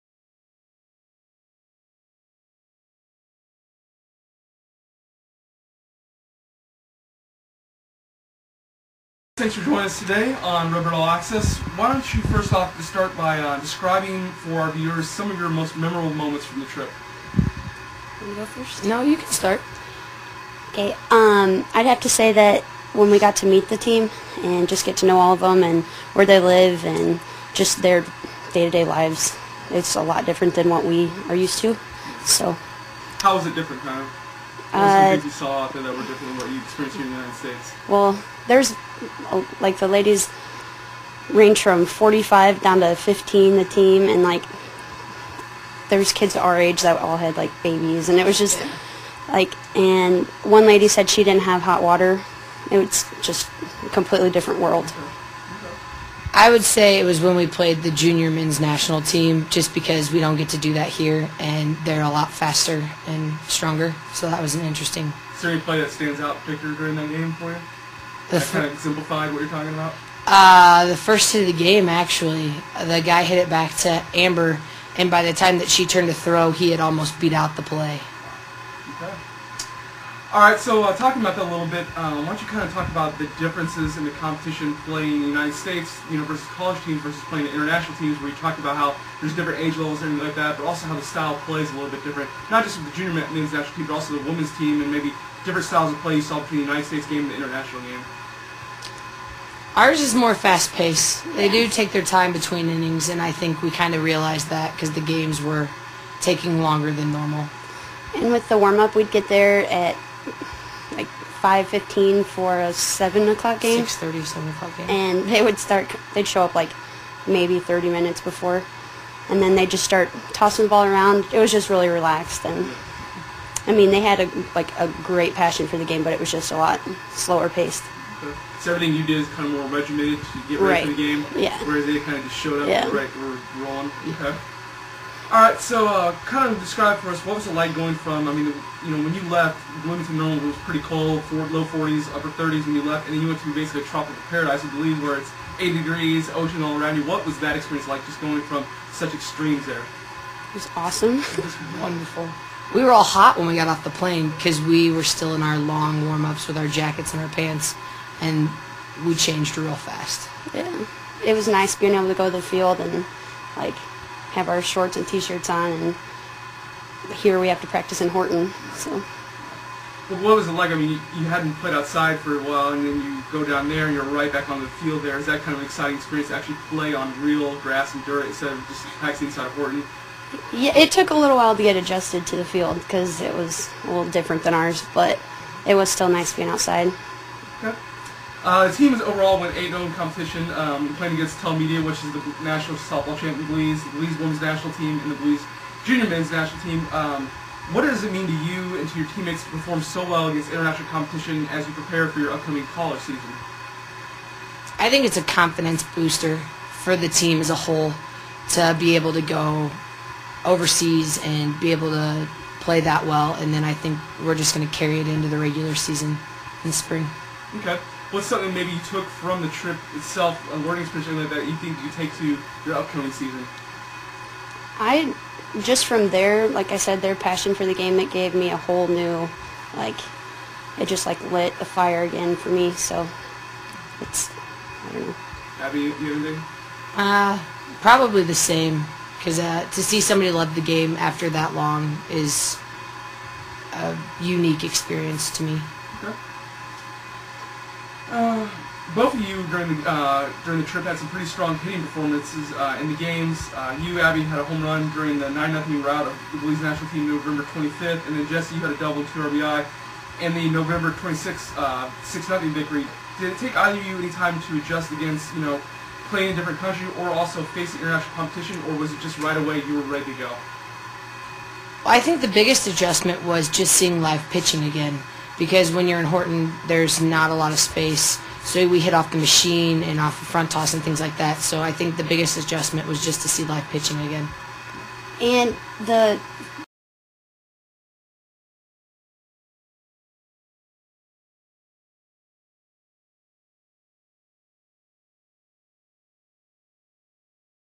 ISU Redbirds: Buker & Olson on Belize
An interview with Illinois State softball players Jessie Buker & Abby Olson.